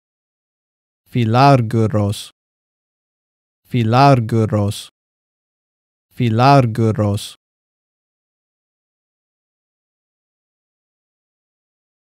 How to pronounce Philargyros in Biblical Greek - (φιλάργυρος / fond of money)
φιλάργυρος (philargyros) means fond of money in Biblical Greek.

This video shows how to pronounce φιλάργυρος in the Erasmian pronunciation system.

Example Bible Verses:

Luke 16:14
Now the Pharisees, who were lovers of money, heard all these things, and they ridiculed him.

2 Timothy 3:2
for people will be lovers of themselves, lovers of money, boasters, arrogant, slanderers, disobedient to parents, ungrateful, unholy,